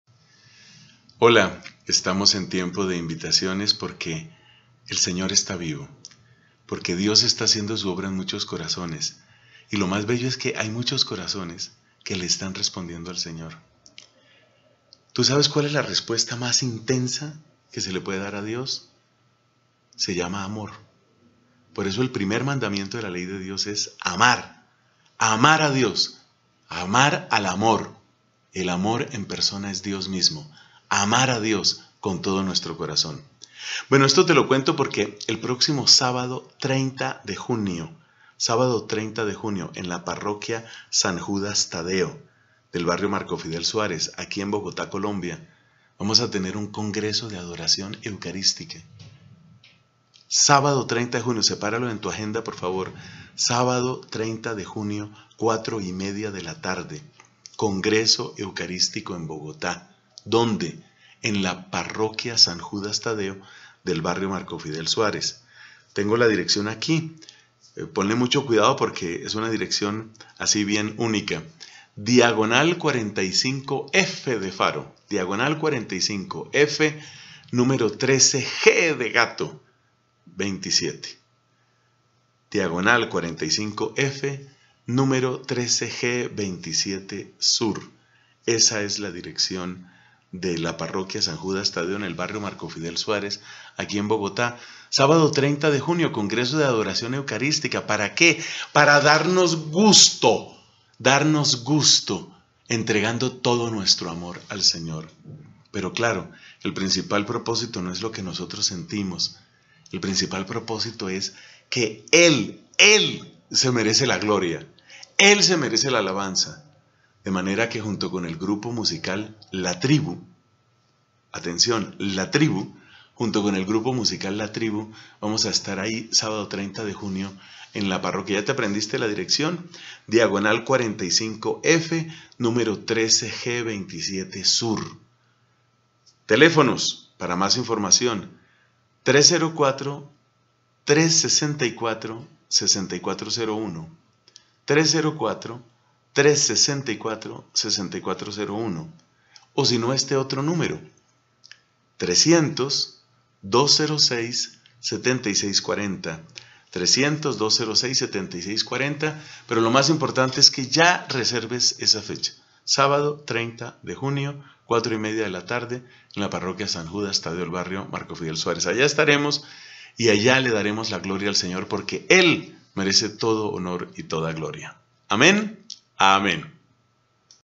Invitación! EVENTO: Congreso Eucarístico en Bogotá, Colombia
CONGRESO EUCARÍSTICO DE ADORACIÓN

Sábado 30 de Junio de 2018, en la Parroquia San Judas Tadeo de Bogotá, desde las 4:30 PM

Dirección: Carrera 45F # 13H-27 Sur, Barrio Marco Fidel Suárez, en Bogotá

Informes: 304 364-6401 ó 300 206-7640

¡Todos invitados! Cupo limitado.